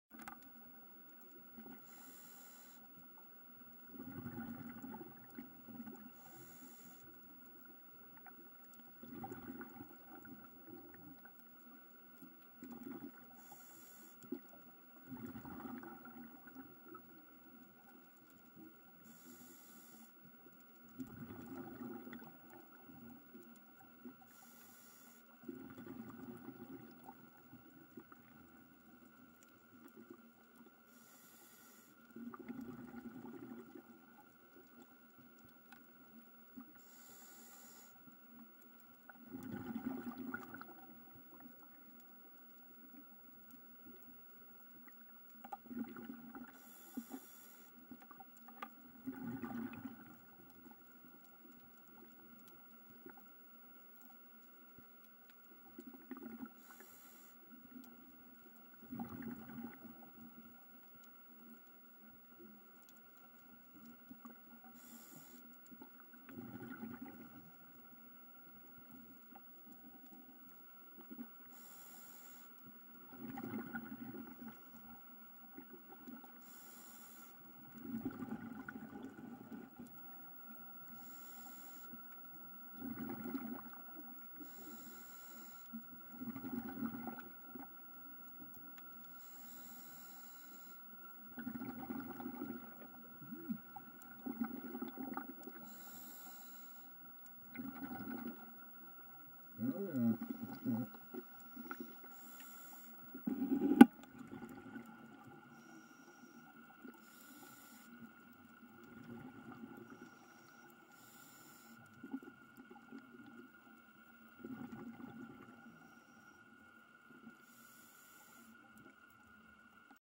Awesome Dive Site
Awesome dive site in Roatan.  At the end the divemaster finds a Lettuce Sea Slug.